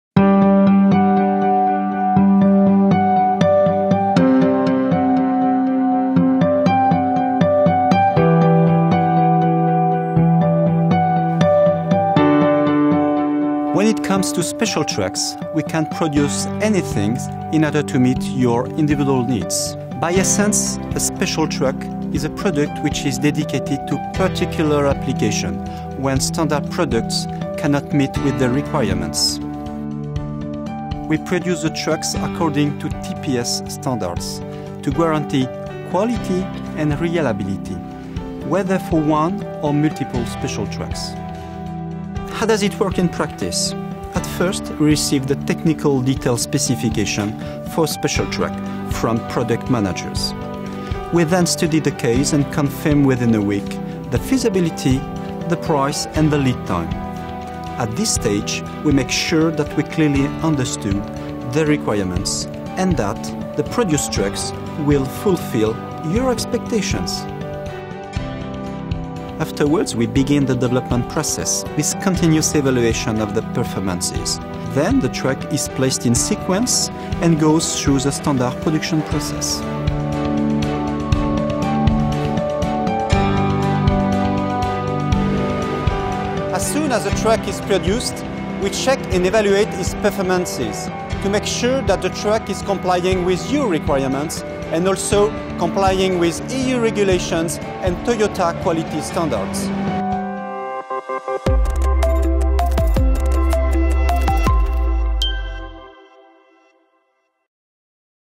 Toyota Material Handling - Produktion af specialtruck
Hos Toyota Material Handling kan vi producere enhver gaffeltruck, du har brug for. Vi bygger specialdesignede truck til at klare særlige opgaver, som ikke kan klares af vores standardprodukter. Med udgangspunkt i en teknisk detaljeringsspecifikation, opretter vi et dybdegående casestudie, producerer trucken og gennemfører i sidste ende udviklingsprocessen ved at evaluere truckens ydelse.

Takket være vores Toyota Production System (TPS), opfylder alle vores special truck EU-reglerne, Toyota's kvalitetsstandarder og naturligvis også dine krav.